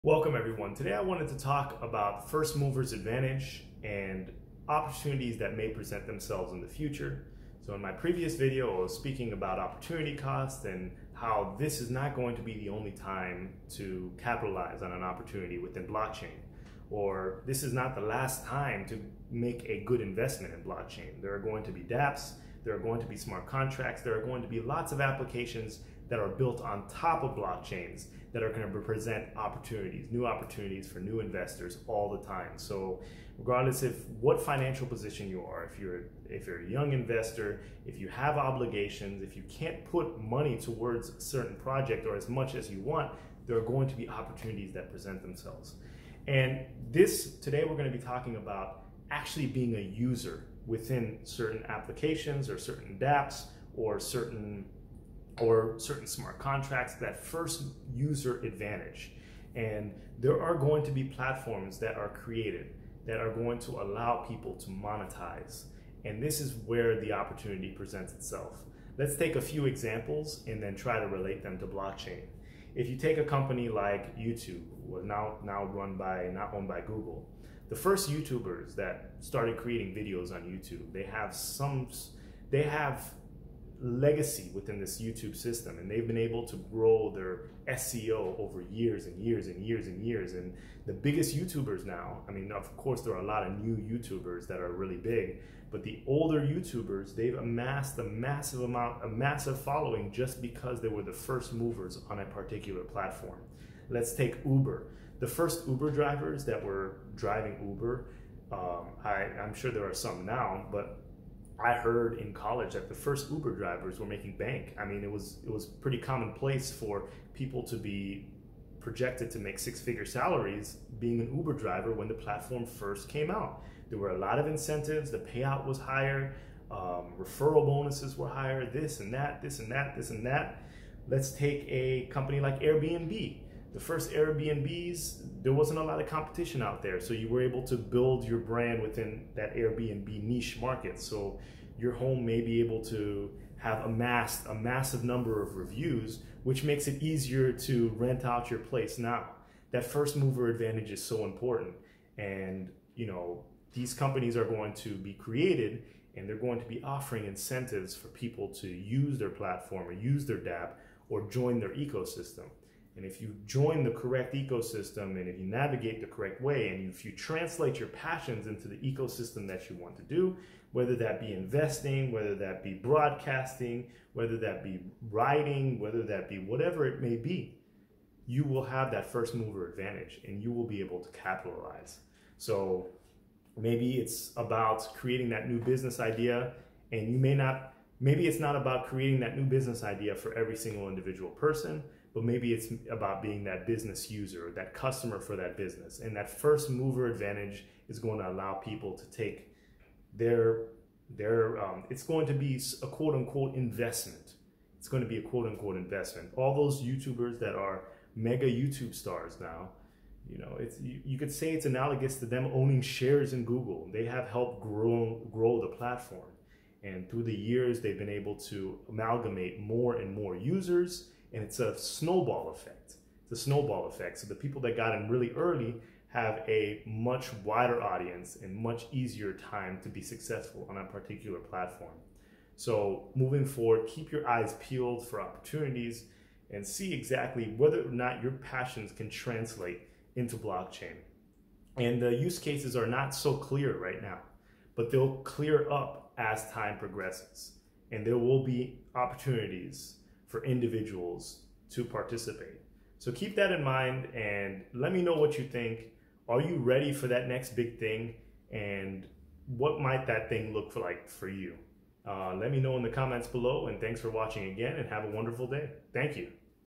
First mover user advantage in blockchain
In this video, we talk about the first mover user advantage in blockchain.

DdzFFzCqrht76dDm8TMCvFqfqvi4XbgrzgXEkkVH2gHoSvmiozz9wfhZEB3j8L4p43qRYF9sZiD2RPCKb9bfTDEZn11VSehUVFDZfgFV